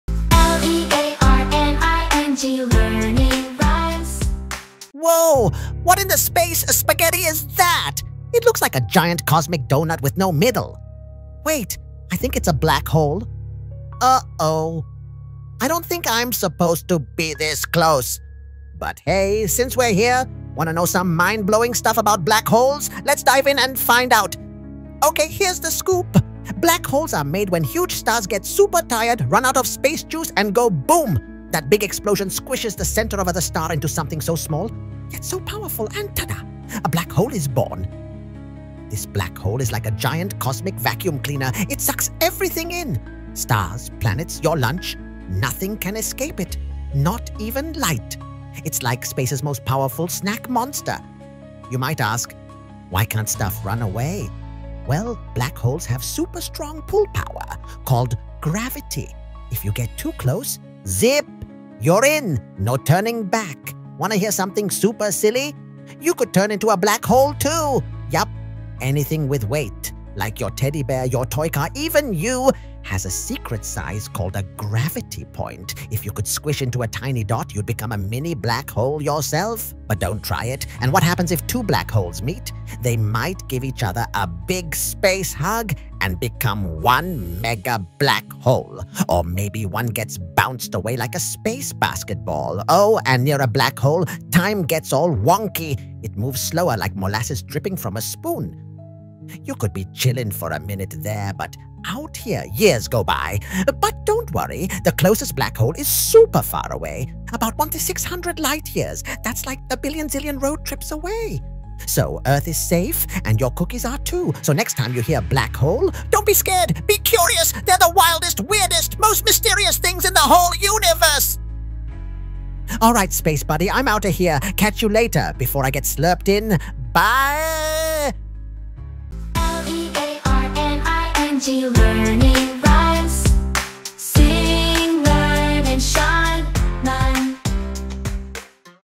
The Super Silly Secret of Black Holes!
Video Script:

Title: “The Super Silly Secret of Black Holes!”

Whoa! What in the space spaghetti is THAT?!
It looks like a GIANT cosmic donut with no middle!
Wait... I think it’s a BLACK HOLE!
Uh-oh… I don’t think I’m supposed to be this close!
But hey, since we’re here…
Wanna know some mind-blowing stuff about black holes?
Let’s dive in and find out!

Okay, here’s the scoop:
Black holes are made when huge stars get super tired,
run out of space juice, and go BOOM!
That big explosion squishes the center of the star
into something so small… yet so powerful!
And ta-da! A black hole is born!

This black hole is like a giant cosmic vacuum cleaner—
it SUCKS everything in! Stars, planets, your lunch...
Nothing can escape it! Not even light!
It's like space’s most powerful snack monster!

You might ask: “Why can’t stuff run away?”
Well, black holes have SUPER strong pull power—called gravity.
If you get too close—ZIP! You’re in, no turning back!

Wanna hear something super silly?
YOU could turn into a black hole too!
Yup! Anything with weight—like your teddy bear, your toy car, even YOU—
has a secret size called a gravity point.
If you could squish into a tiny dot,
you’d become a mini black hole yourself! (But don’t try it!)

And what happens if TWO black holes meet?
They might give each other a big space hug—
and become one MEGA black hole!
Or maybe one gets bounced away like a space basketball.

Oh, and near a black hole, time gets all wonky!
It moves slower, like molasses dripping from a spoon!
You could be chillin’ for a minute there,
but out here—YEARS go by!

The closest black hole is SUPER far away—
about 1,600 light years! That’s like…
a billion zillion road trips away!
So, Earth is safe, and your cookies are too.

So next time you hear “black hole,”
don’t be scared—be CURIOUS!
They’re the wildest, weirdest, most MYSTERIOUS things in the whole universe!

Alright, space buddy, I’m outta here!
Catch you later… before I get slurped in!
BYEEEEE!

The Super Silly Secret of Black Holes”, a fun and educational space adventure for kids!  In this animated video, a goofy octopus scientist takes children on a journey to learn amazing facts about black holes. We’ll discover how black holes form from giant dying stars that go boom (supernova)
, why black holes act like cosmic vacuum cleaners gobbling up everything (even light!)
, and what would happen if you got too close to one.  Through kid-friendly language and playful visuals, we explain mind-blowing concepts like gravity and time slowing down near a black hole
. Don’t worry, the Earth is completely safe – the closest black hole to us is 1,600 light years away
 (super far!).

Learning Rhymes®, all the characters, and logos used are the registered trademarks of Learning Rhymes Studios."